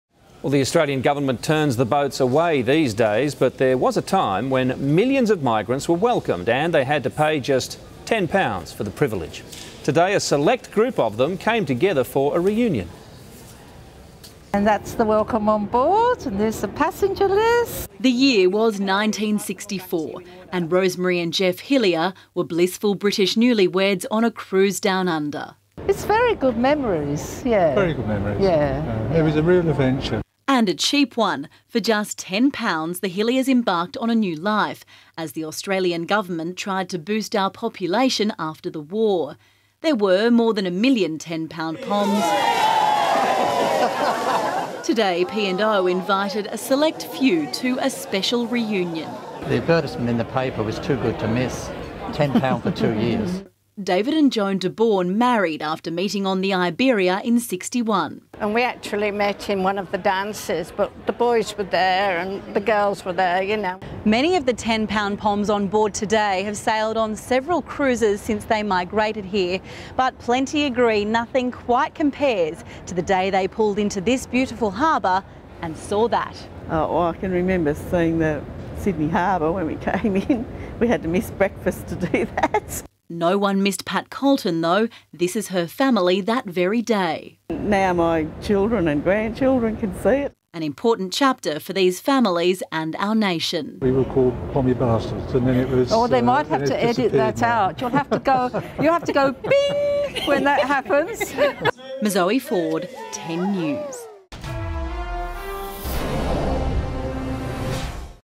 Aussies at heart
A select group of migrants that paid just ten pounds to come to our shores have celebrated a special reunion.